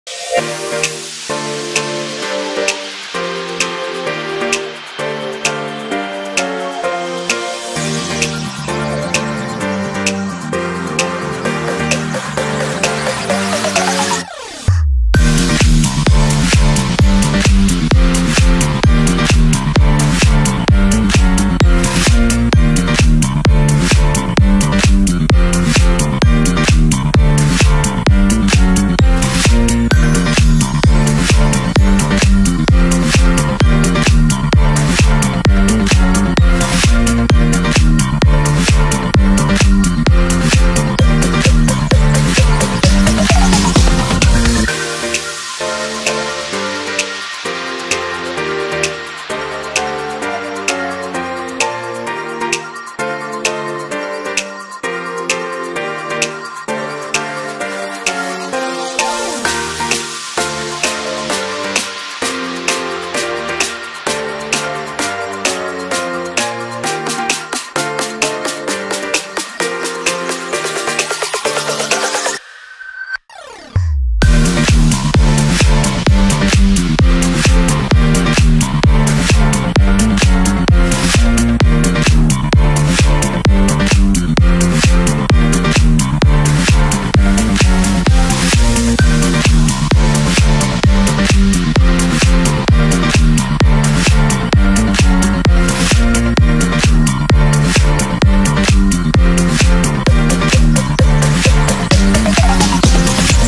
2D ANIMATION SHOWREEL 2022 | Taqibunn!
A compilation of my best works throughout 2021 to 2022.